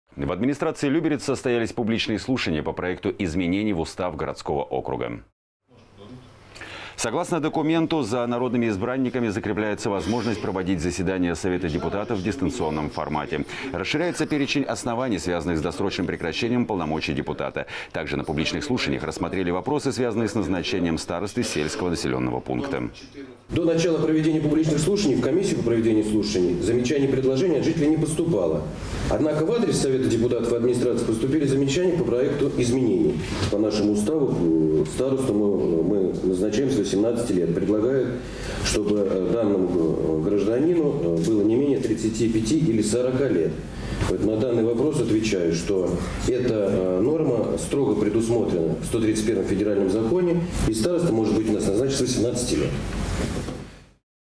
В устав г.о. Люберцы внесли изменения
В администрации Люберец состоялись публичные слушания по проекту изменений в устав городского округа.

Согласно документу, за народными избранниками закрепляется возможность проводить заседания Совета депутатов в дистанционной форме. Расширяется  перечень оснований, связанных с досрочным прекращением полномочий депутата. Также на публичных слушаниях рассмотрели вопросы, связанные с назначением старосты сельского населенного пункта.

Михаил Криворучко, заместитель главы администрации г.о. Люберцы:
 «До начала проведения публичных слушаний в комиссию по проведению слушаний замечаний и предложений от жителей не поступало. Однако в адрес Совета депутатов поступили замечания по проекту изменений. По нашему у ставу старосту мы назначаем с 18 лет, предлагают, чтобы данному гражданину было не менее 35-40 лет. На данный вопрос отвечаю, что эта норма строго предусмотрена 131 ФЗ и старостой можно быть назначенным с 18 лет».